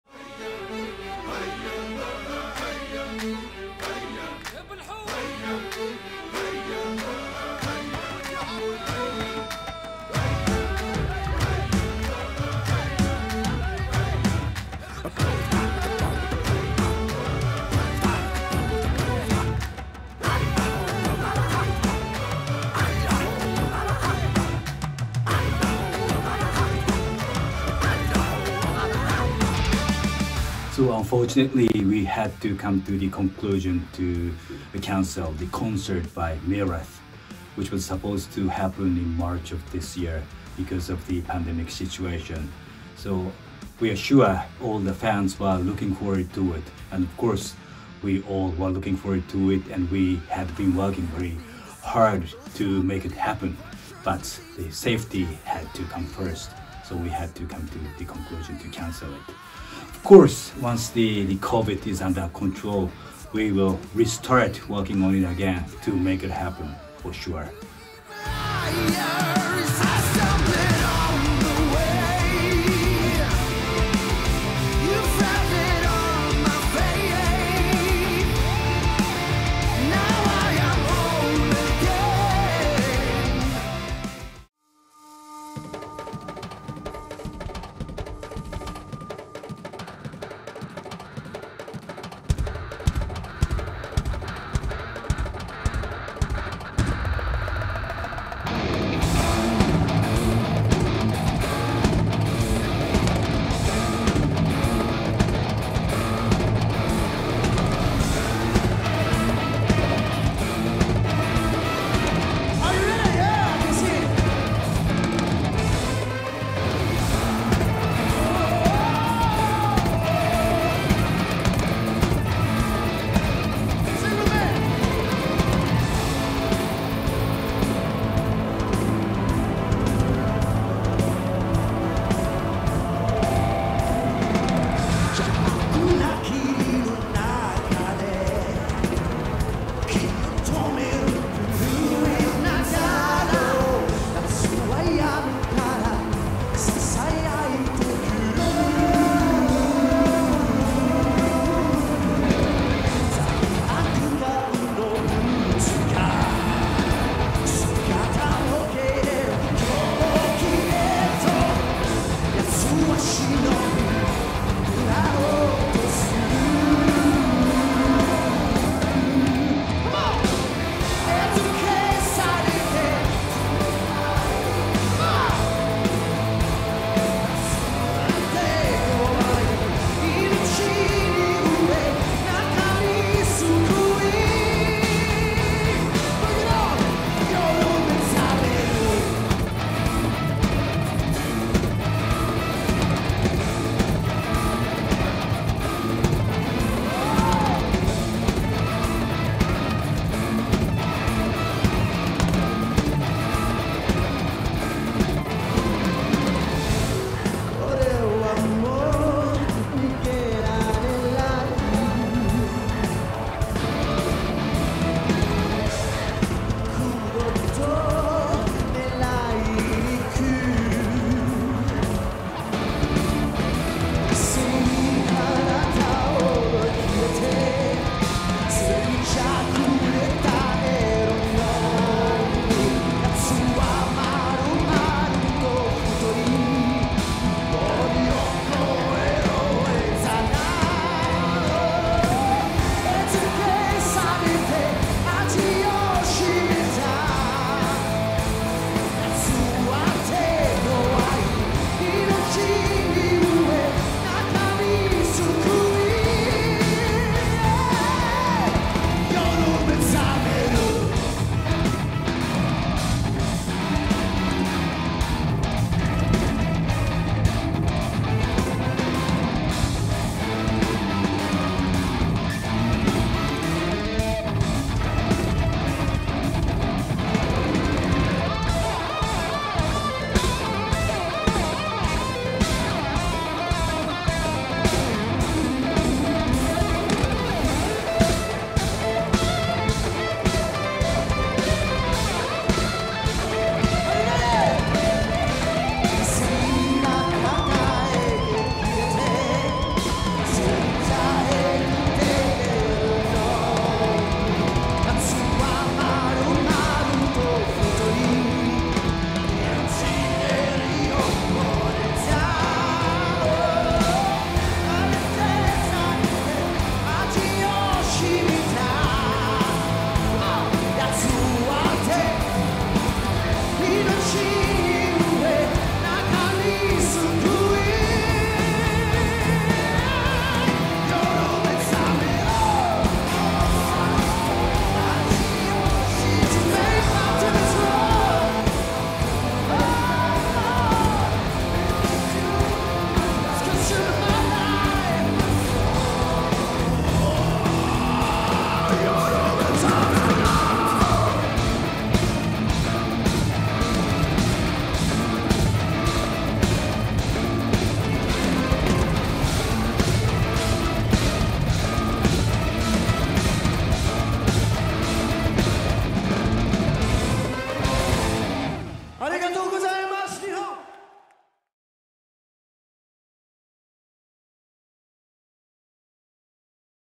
【J-LODlive2 キャンセル】2021年3月20日(土)/21日(日) 渋谷ストリーム ホール ミラス来日公演
【公演概要】
2021年3月20日(土)/21日(日) 渋谷ストリーム ホール / 出演：ミラス
MYRATH THE MAGICAL TOUR 2020 IN JAPAN(振替公演)は、国内外の新型コロナウイルス(COVID-19)感染拡大と、海外からの渡航が制限されている状況を考慮し、中止となりました。